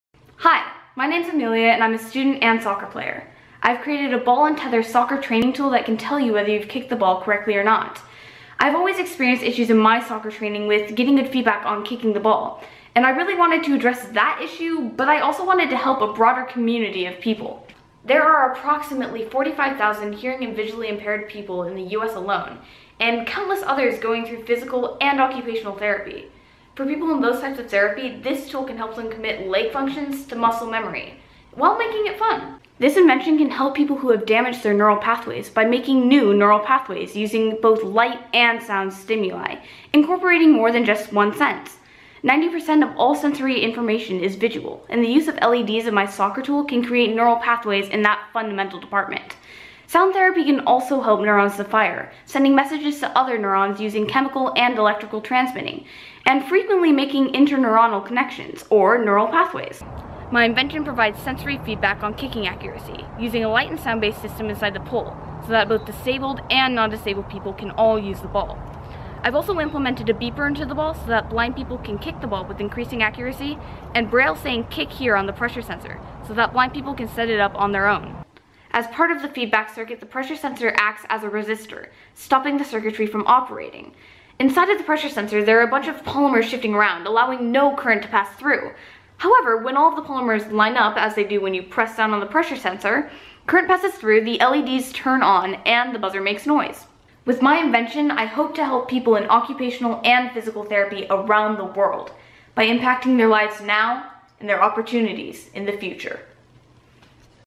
2016 National Finalist: Amelia Day
Watch the entry video that earned Amelia Day place as a 2016 Young Scientist Challenge Finalist!